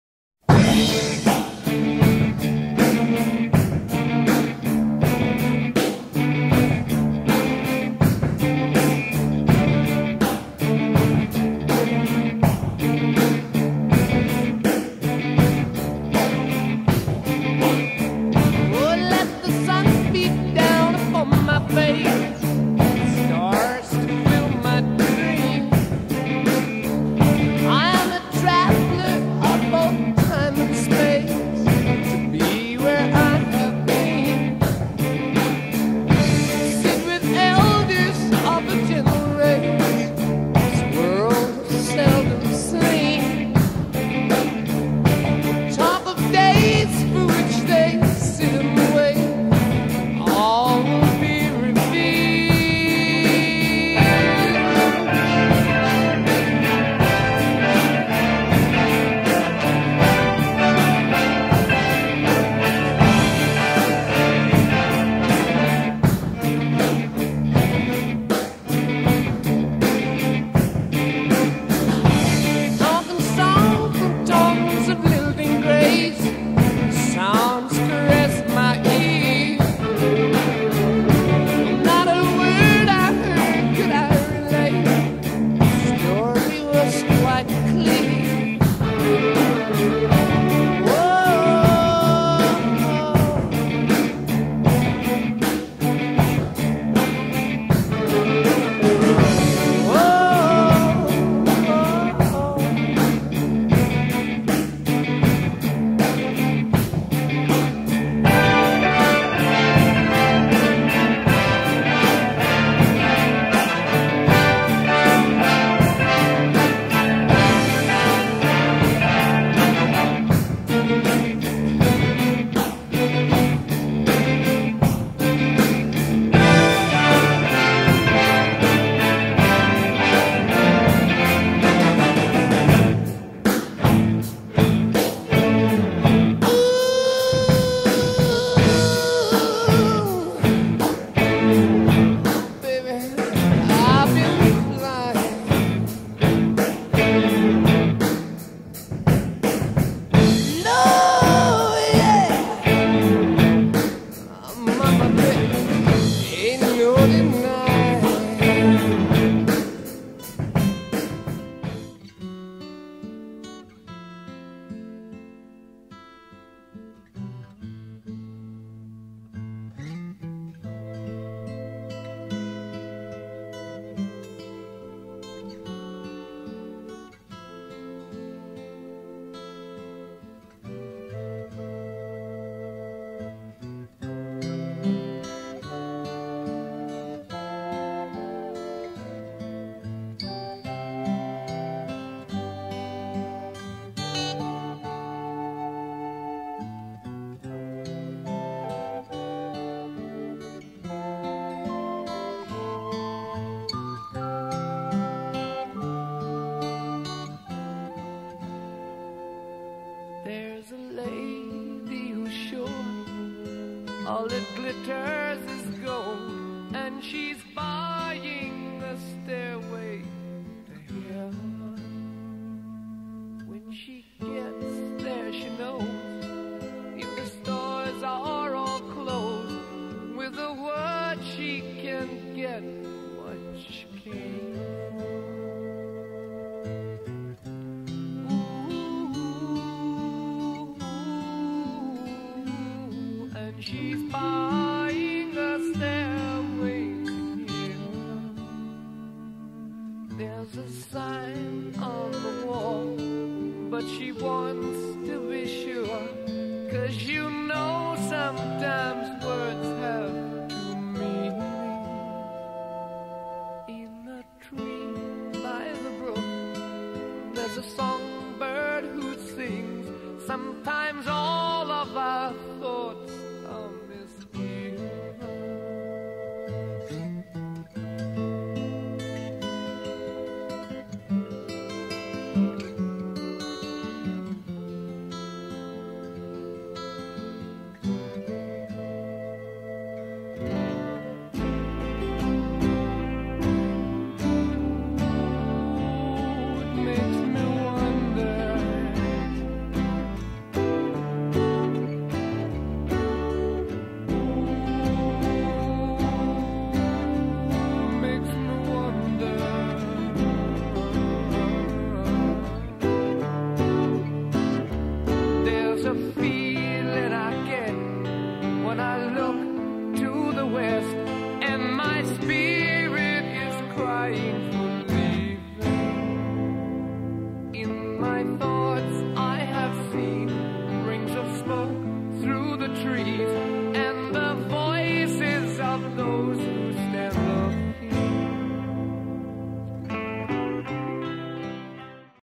Frontman Robert Plaint ( Led Zeppelin) musica - Led Zeppelin
Foto frontman Robert Plaint, musica Led zeppelin, della pagina VideoradiostarPage e Radiomarina DJ gruppo di Facebook di Marina La Fenice